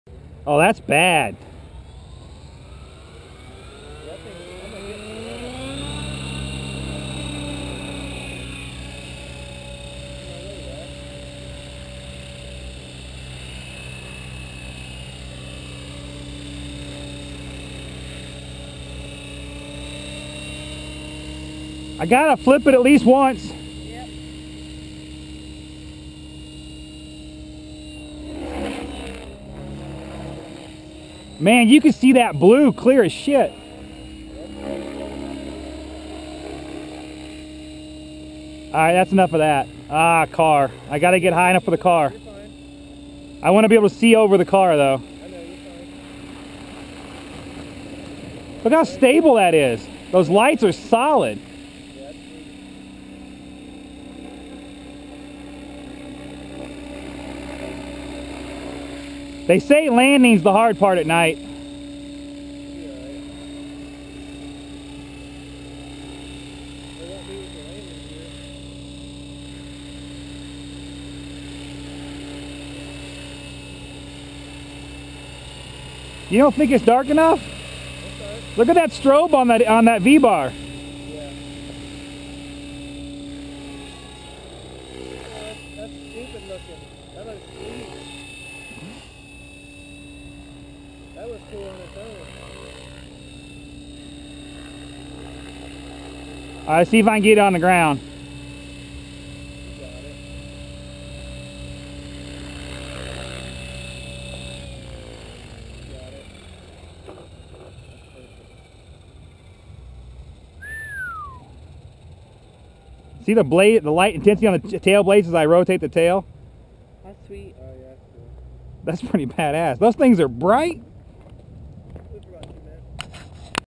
Synergy E7 - Night Flight test - Dark
Doing a test of the Edge 693 FBL Night Lites blades and Rotor Tech Luminous 106 tails.

These things are very bright! Next video will have the canopy and boom lights installed.